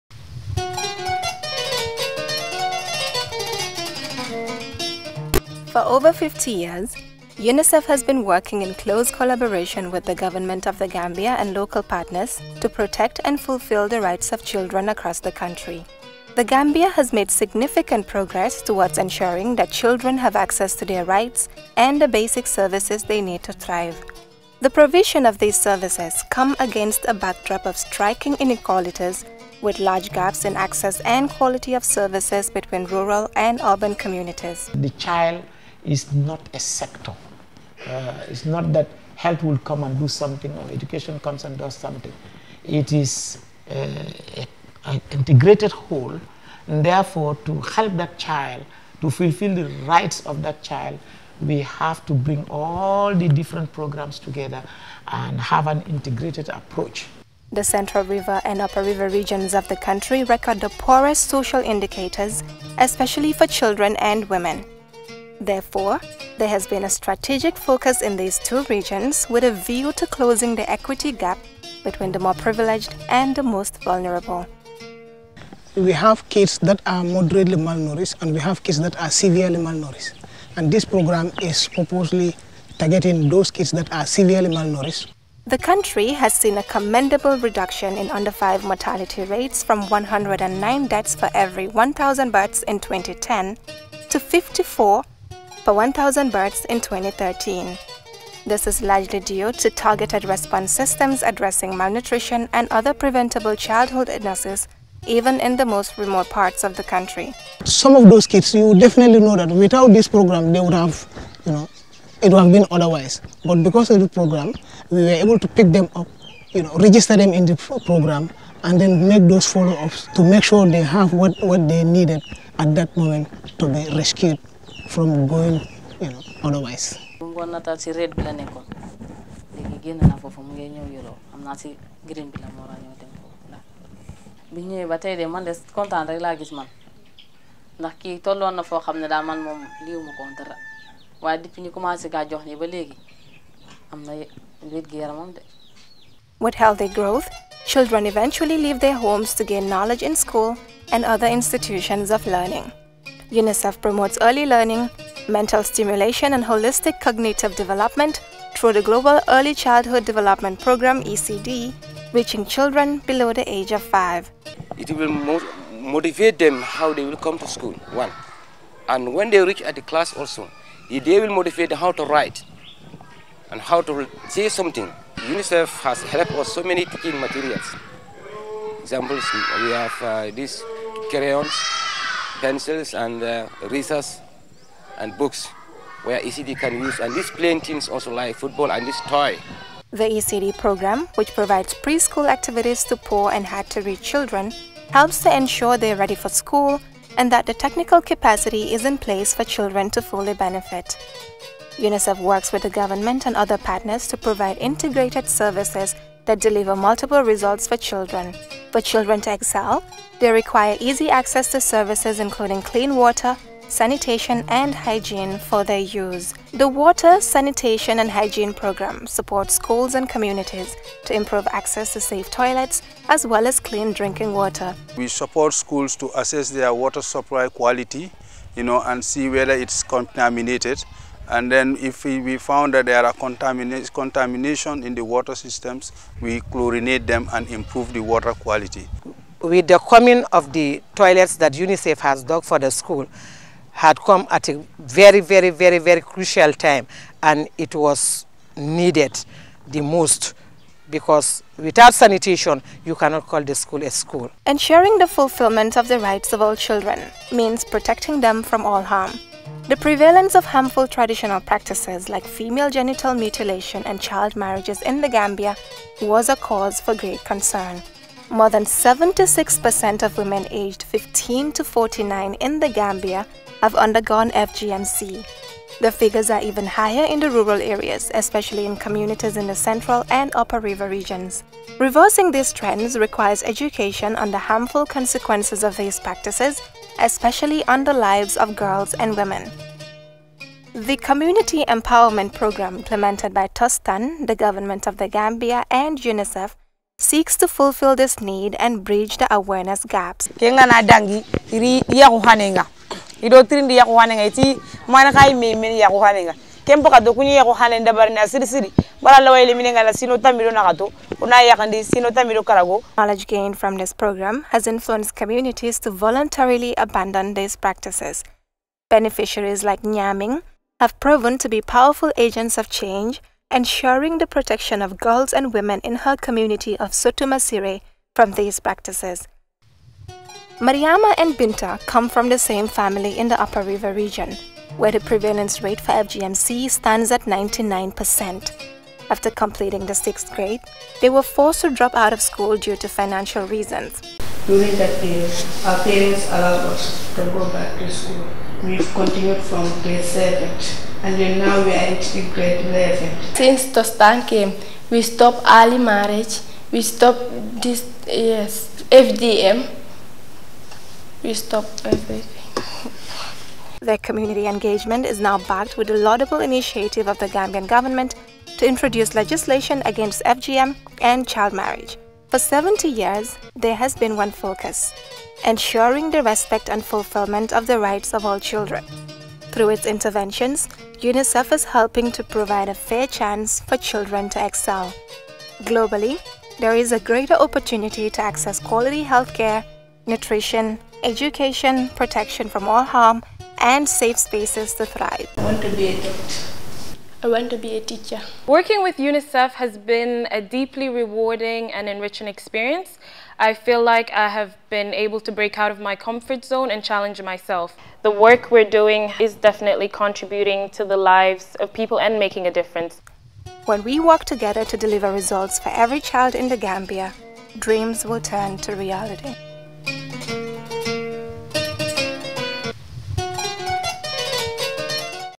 UNICEF @ 70 DOCUMENTARY
UNICEF has been working in The Gambia for over 50 years, in close collaboration with the government of The Gambia and local partners, to promote and protect the lives of children at national and area based levels.